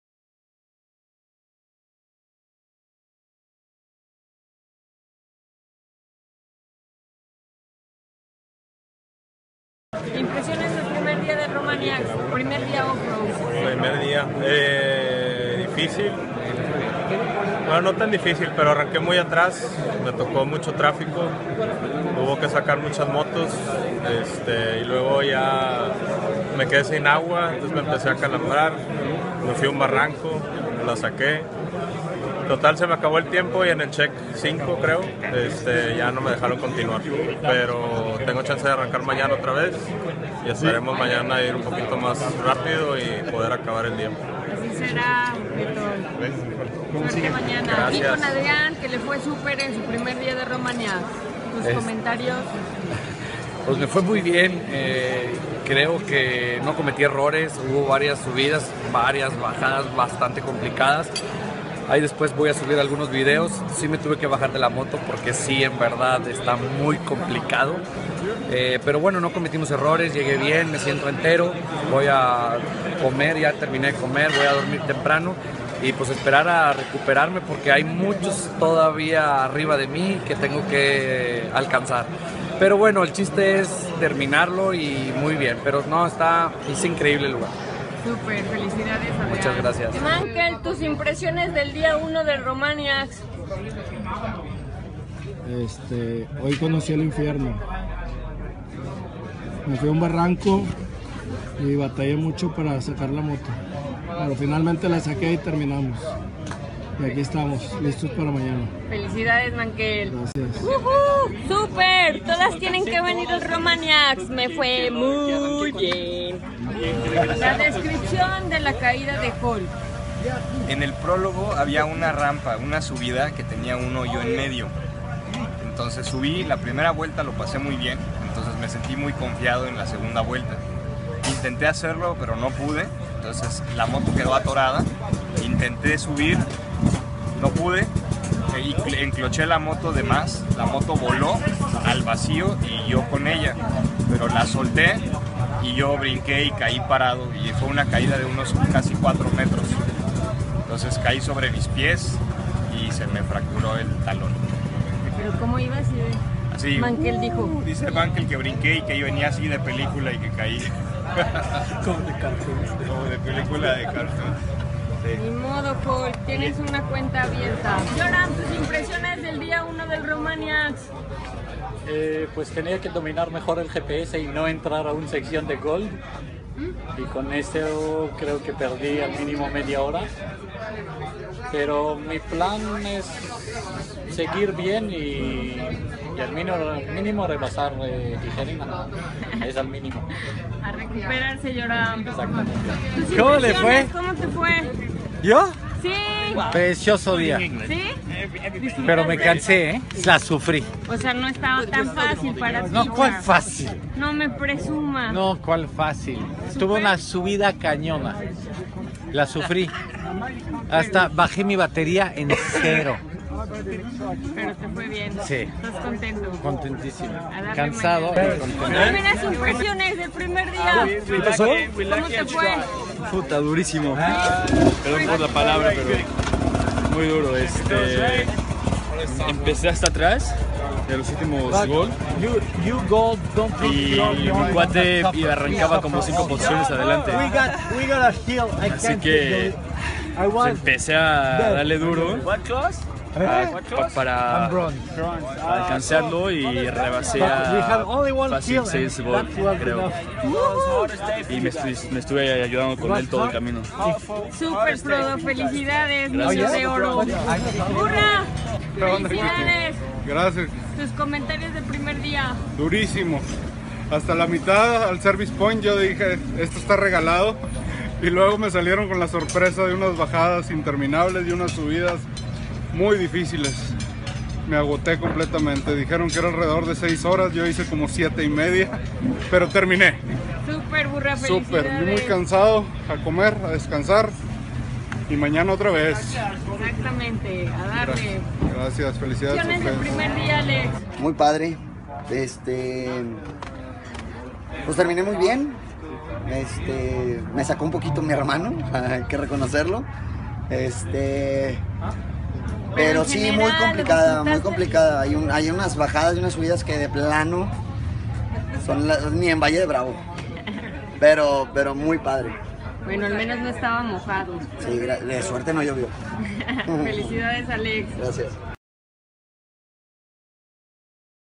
Mexicanos en Red Bull Romaniacs 2015 Día 1, Hard Enduro
Los pilotos mexicanos que participan en el Hard Enduro, Red Bull Romaniacs, nos platican como fue su primer día de montaña. Jesús Zavala, Roberto Quintanilla, Didier Goirand, Yoram Lavee, Irma Cristina Silva, Mankel Morales.